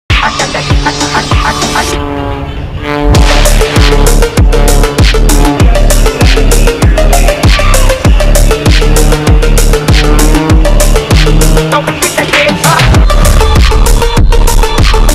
Glenn Graham testing the laws of physics
Glenn Graham flying the @paulbennetairshows Zivko Edge 540.